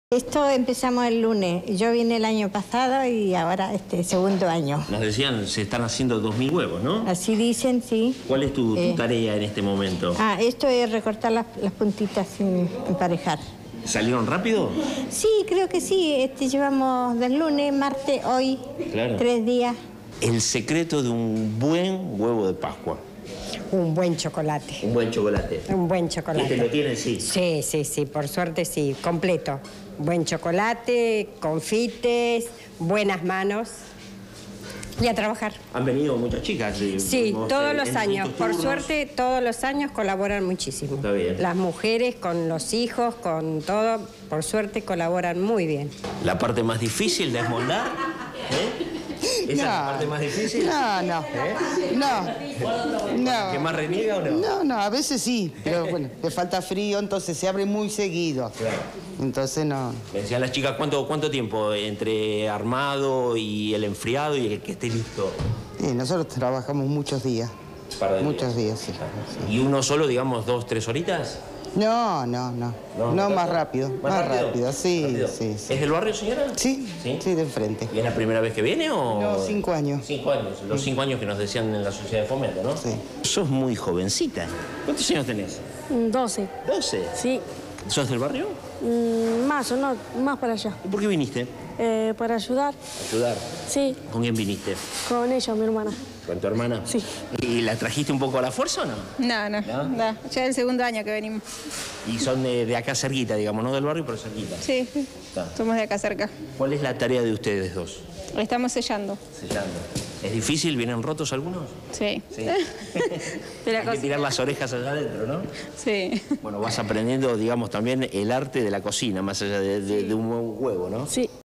¡2.000 huevos de pascua!
Se prepararon para los vecinos de Maldonado